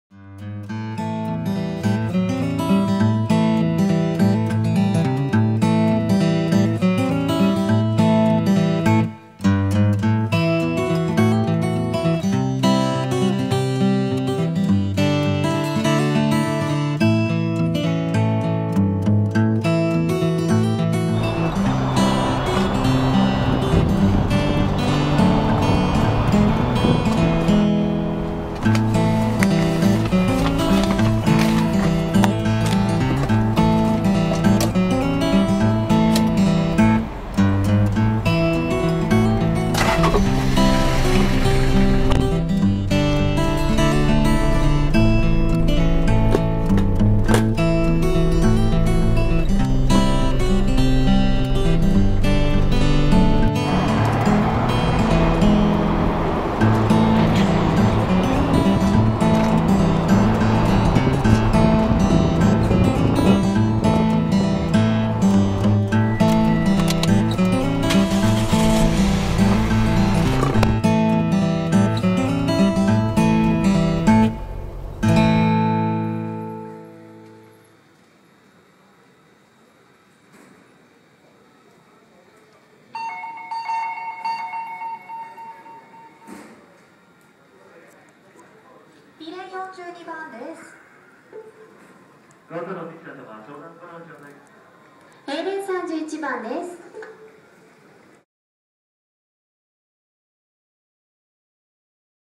Japanese Dealer Auction
So many Japanese Cars (and some European Cars) are sold every day at Japanese Car Dealer Auctions.  You can find any type of cars here.